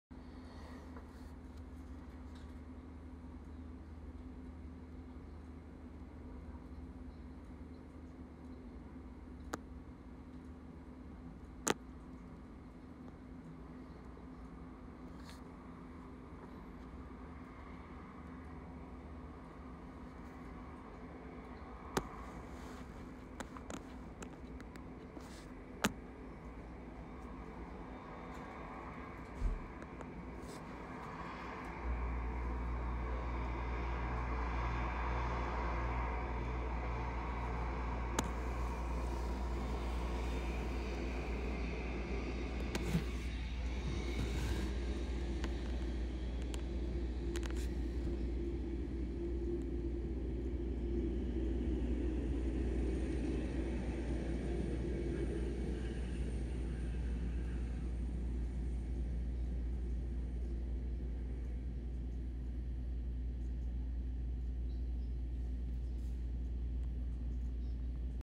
Returning from customs flight test on Boeing 787 airplanes
Completing custom flight test for Boeing 787 airplanes.

daily dose of aviation
#737 max 10
paris air show
paris air show 2023
carves minecraft